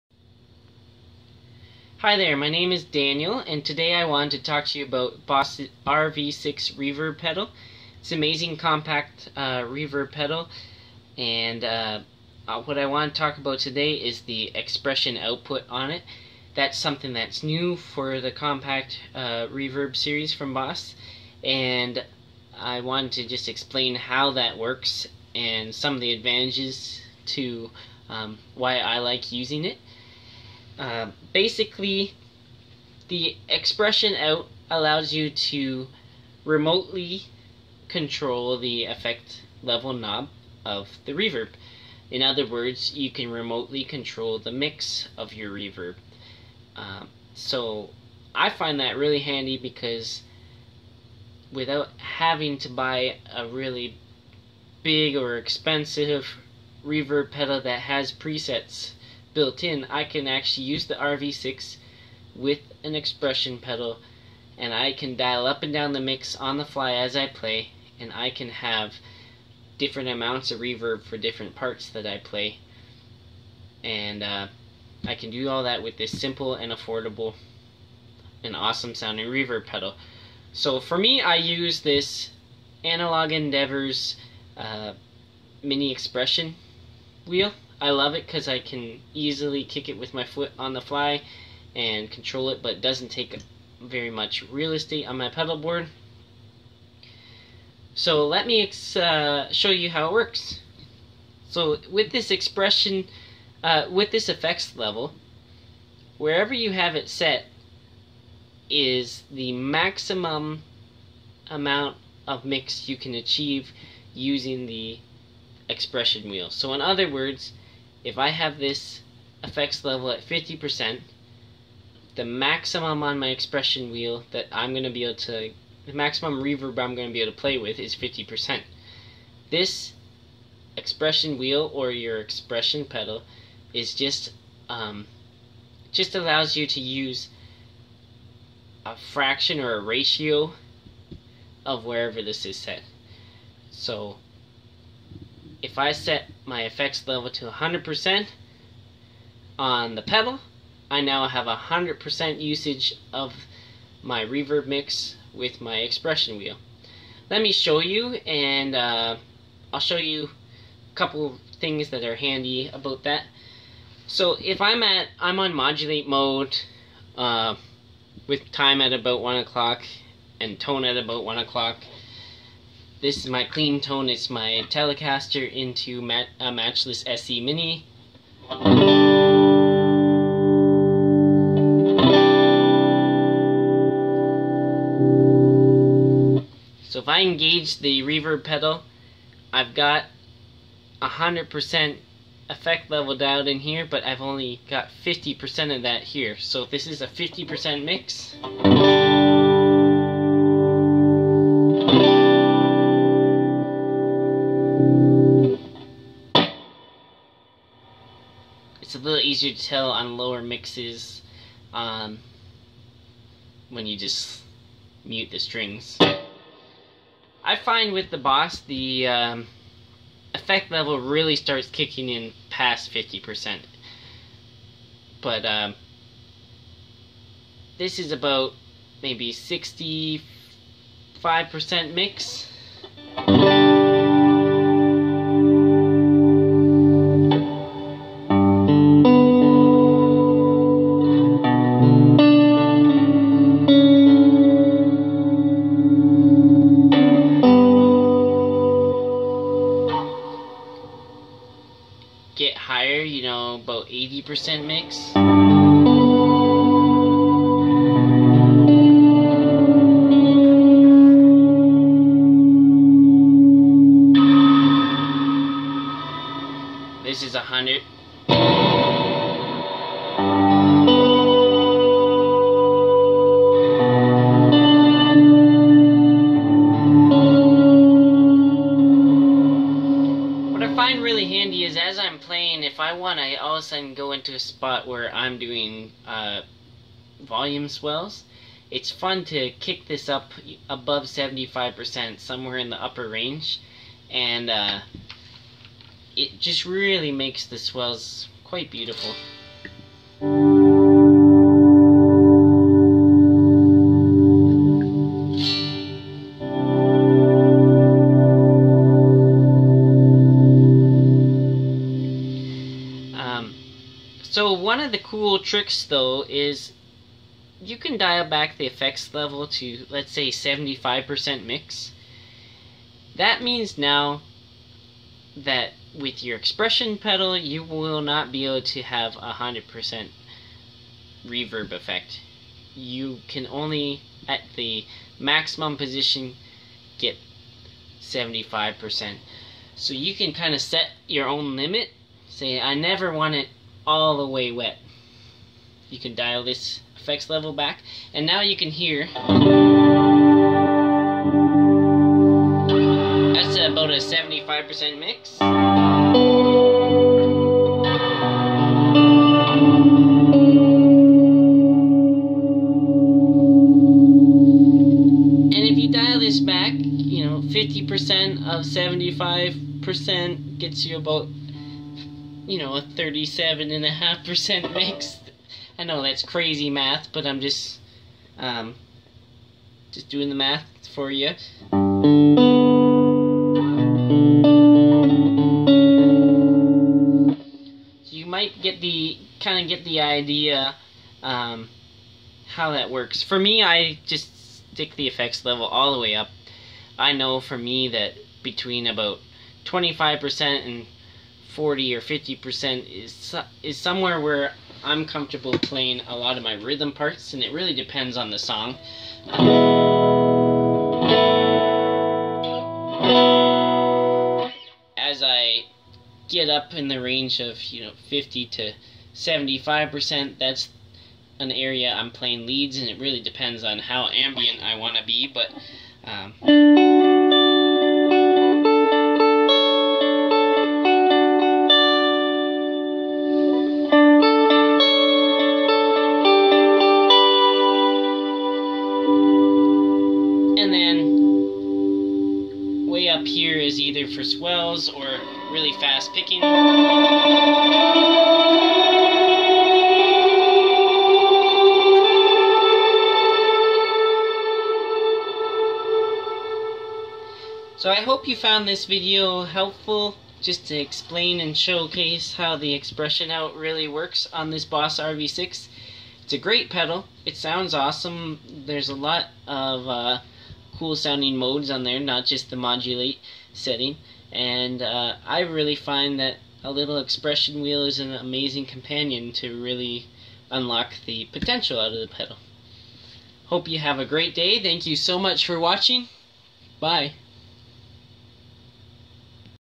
Boss RV6 - explaining the expression control
In this video I talk about and demonstrate the expression output on the Boss RV6 reverb pedal. I am using the Analog Endeavors mini EXP wheel.